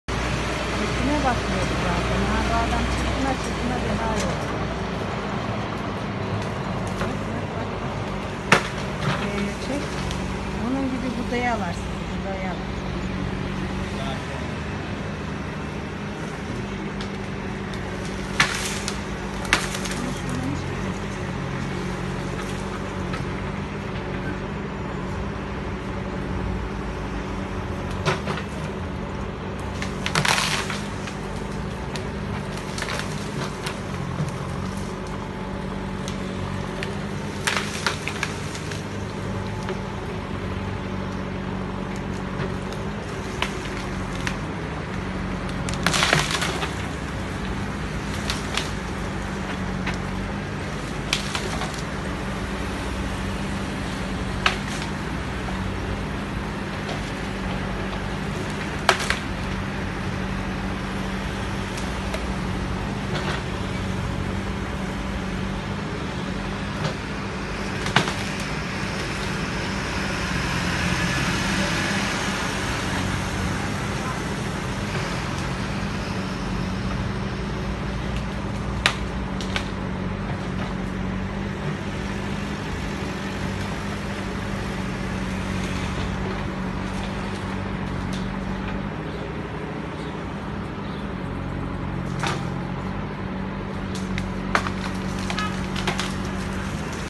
Park ve Yürüyüş Yolu çalışmalarında, 65 yıllık çam ağacını kestiler
İstanbul Büyükşehir Belediyesi’nin Zeytinburnu Zübeyde Hanım Caddesi üzerindeki Marmaray hattının hemen yanında başlattığı park ve yürüyüş yolu düzenlemesi çalışmalarında, 65 yıllık çam ağacını kesmesi vatandaşların tepkisine neden oldu.